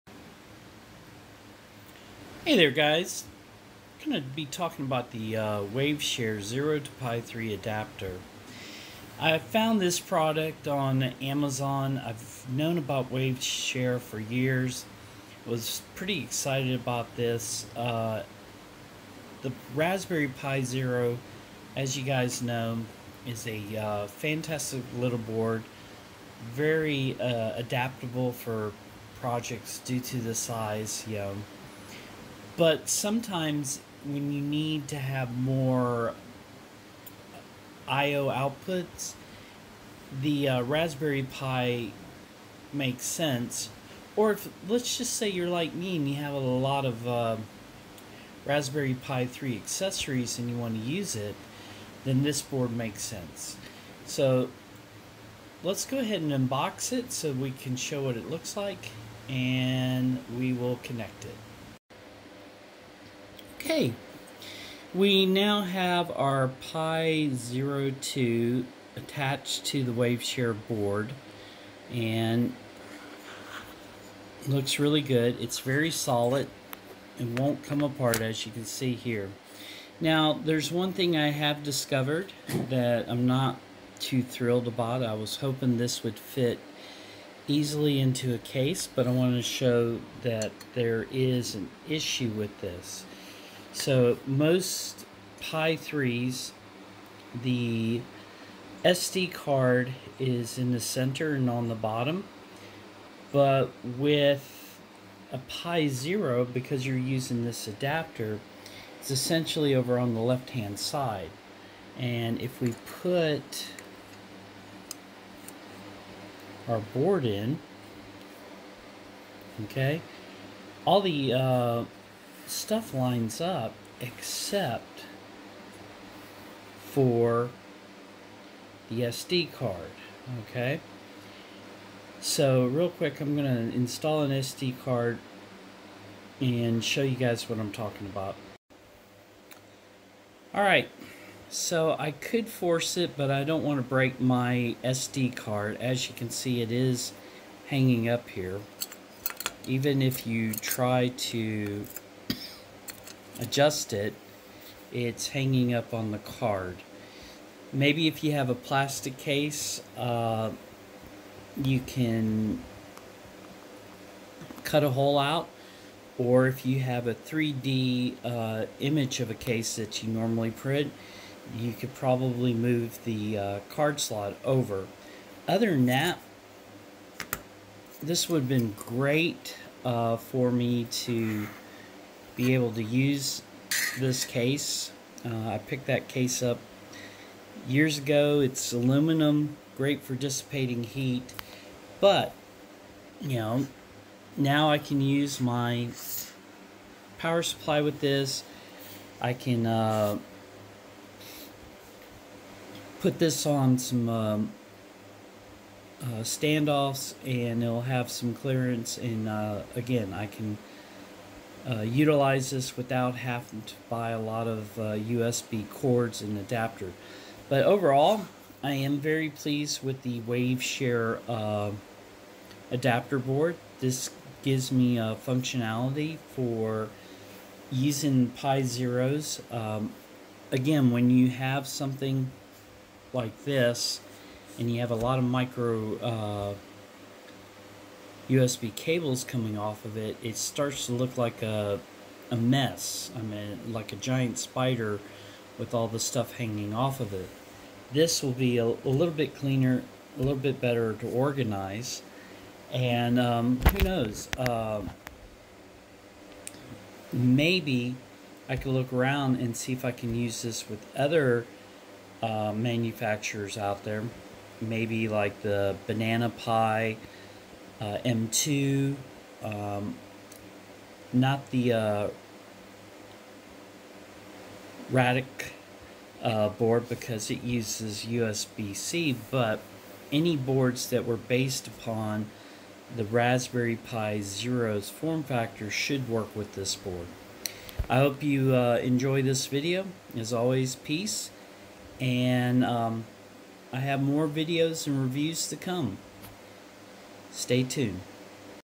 Pi Zero to Pi3 Adapter (A) from Waveshare.
If you are like me, you have a few Pi Zero laying around, I wanted to be able to use them and have more USB ports and Ethernet that were easy to use, and didn't look like a spider with all the cords hang off them.

Waveshare has two model of adapter that made it possible to turn your Pi Zero into a Pi3, the problem most Pi 3 cases won't work.

This video show the A version of the Adapter.

This would work best with open cases.

Wiki on the board

Version B Board

Case I recommend for the board(s)